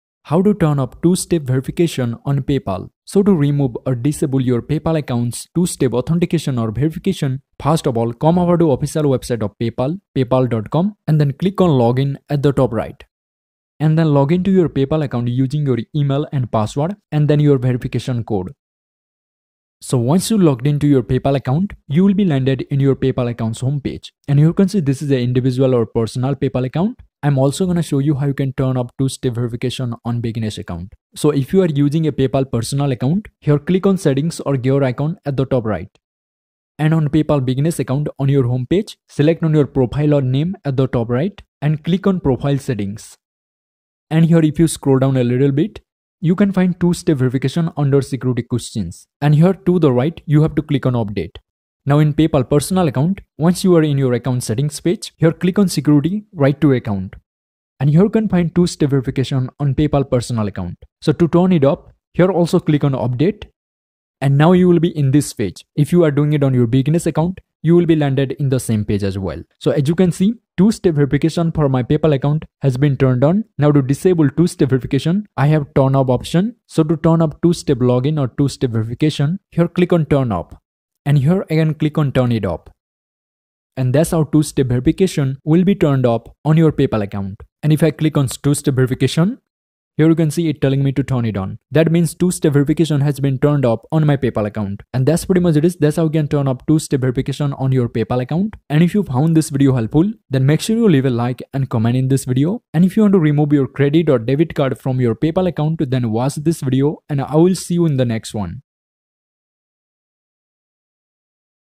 How to Turn Off Two-Step Verification on PayPal | Turn Off Paypal 2FA
How to disable or turn off 2-step verification on PayPal Account

In this tutorial I'll show you how you can remove two step authentication or verification from your paypal account so that whenever you sign in to paypal you'll not be required to use the verification code. So if you don't know how to turn off or remove two factor authentication on PayPal, then this video is going to be very helpful for you. Make sure you watch this video all the way to the end and you can easily turn off Paypal account 2FA or 2SV.